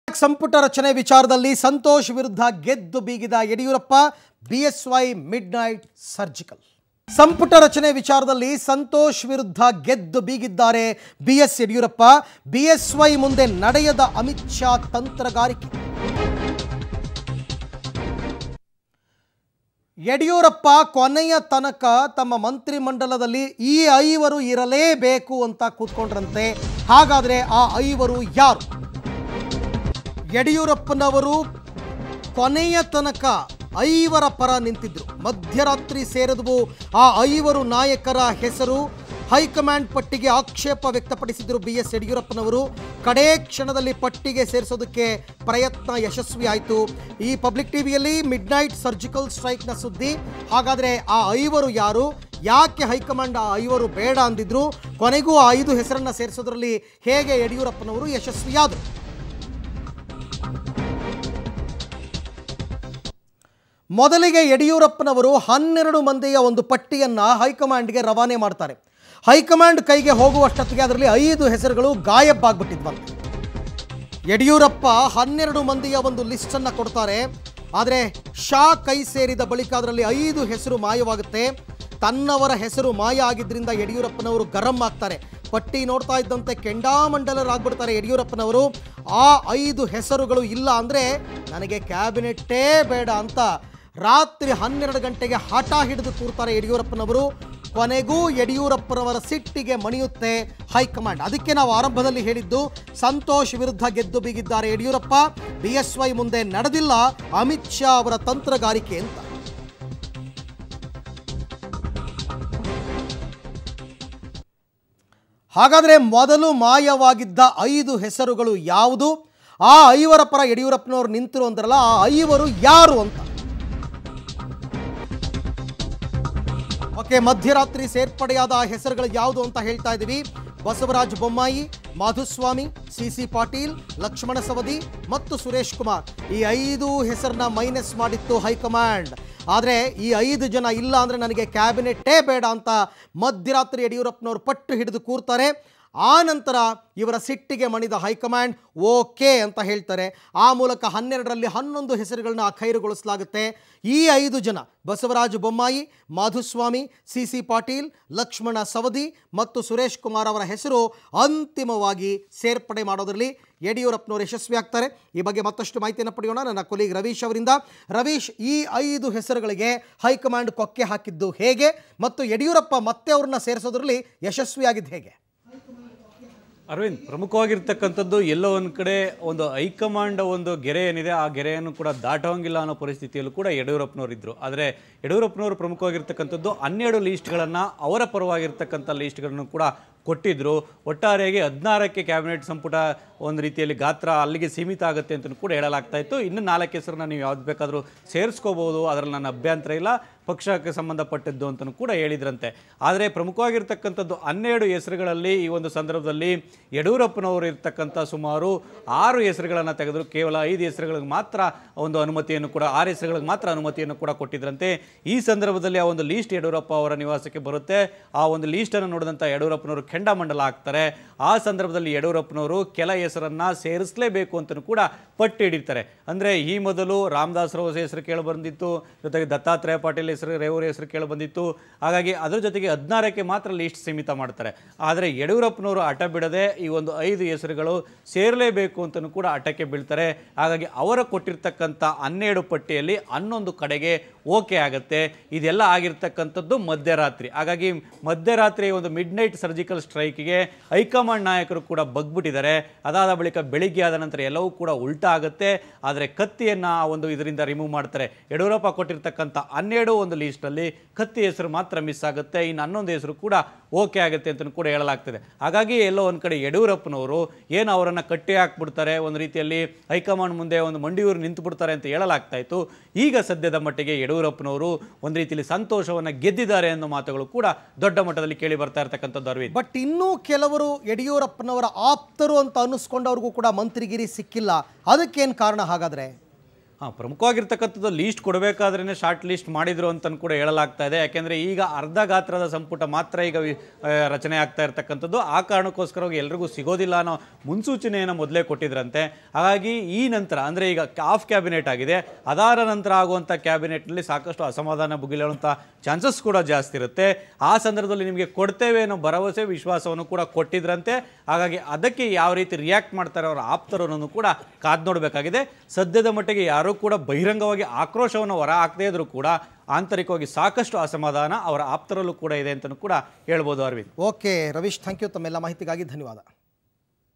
Yeddyurappa 'Midnight Surgical Strike' | BSY Makes Changes To Amit Shah's List Of Ministers
Yeddyurappa 'Midnight Surgical Strike' | Yeddyurappa received the ministers' list yesterday night from Amit Shah. But, Yeddyurappa was unhappy with the list as the names of his close aides were missing. Yeddyurappa has spoken to Amit Shah in the midnight and has included all his close aides names in the list.